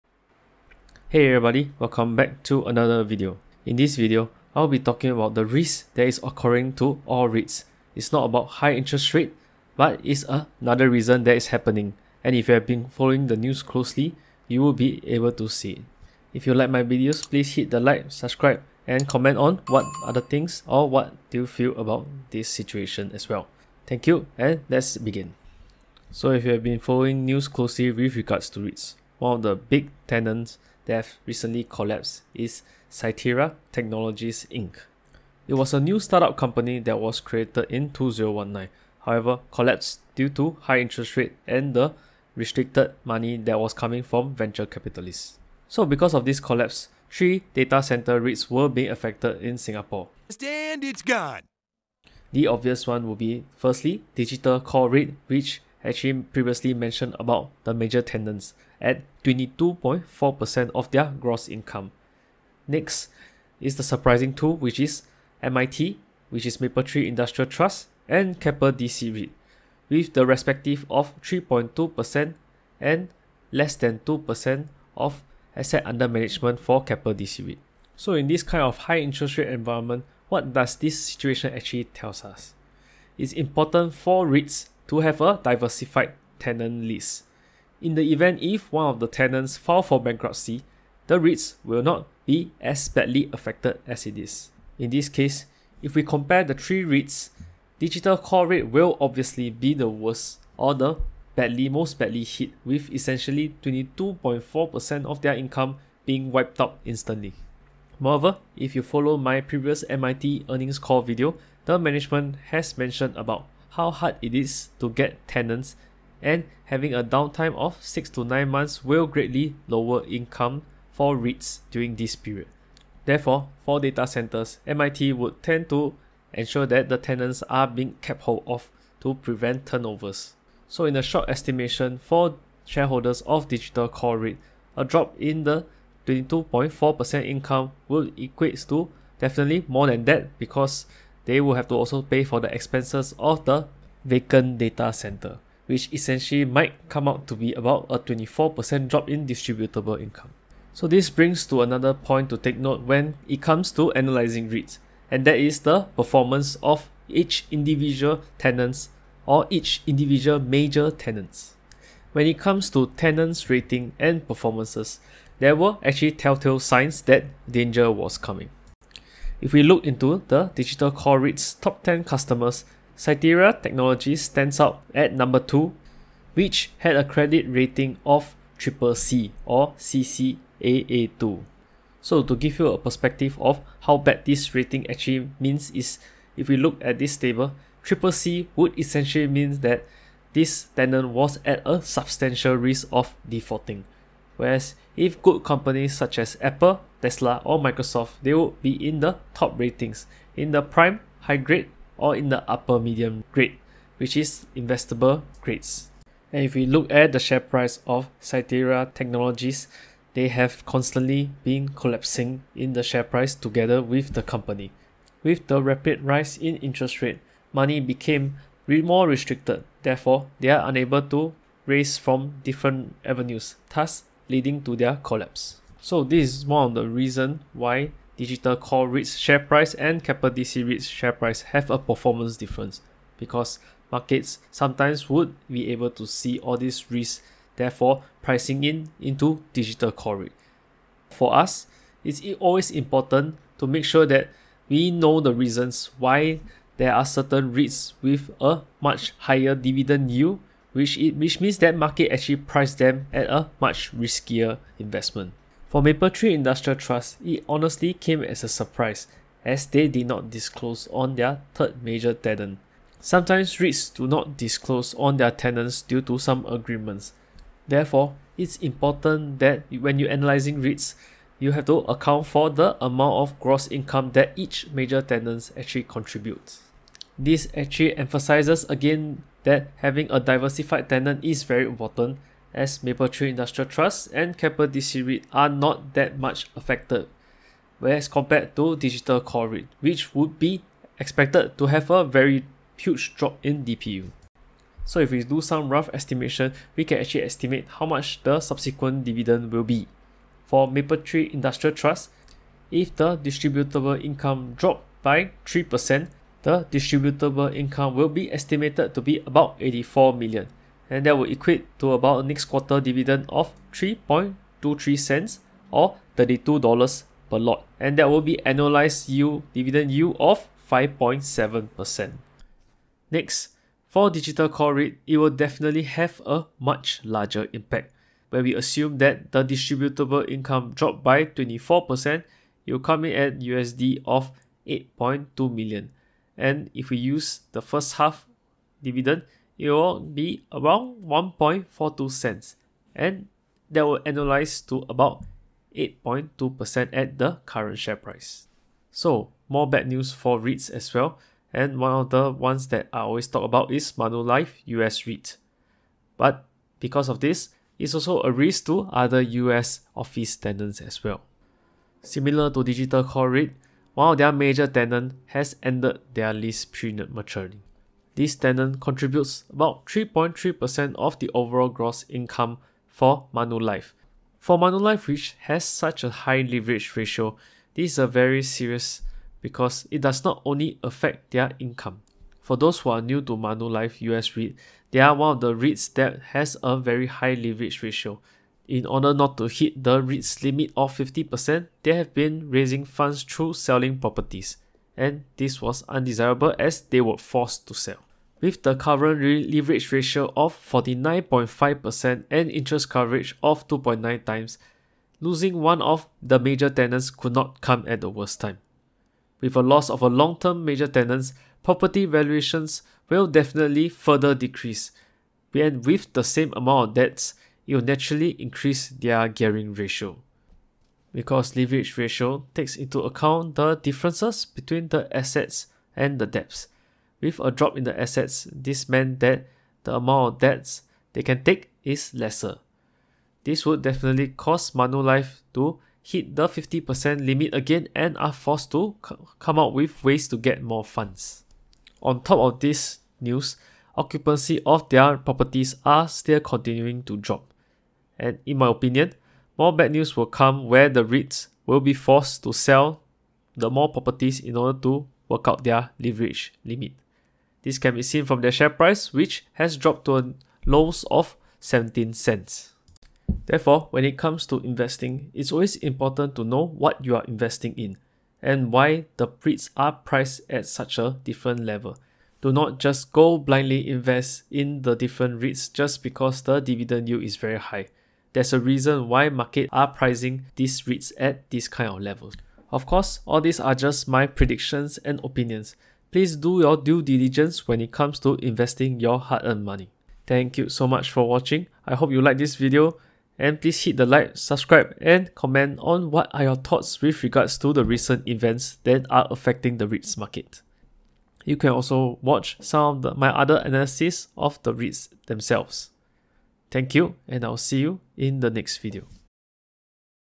Risk for Dividend investing in Reits
With the Federate Reserve deciding to stop interest rate hikes and inflation data is dropping, it brings about optimism for Reits to make a good comeback. However, there are other risk roaming around for all the Reits regardless of which country or region they are in.

In this video we'll look at the examples of the impacts of Digital Core Reit(DCUR), Mapletree Industrial Trust(ME8U), Keppel DC Reit(AJBU) and Manulife US Reit(BTOU).

This video is for entertainment and educational purposes. Everything is based on my opinion and would definitely recommend become to do their own research before making any investment decisions

0:00 Introduction
0:25 Tenant Default
2:29 Performance of Tenants
5:50 Manulife Reit Disaster
7:44 Conclusion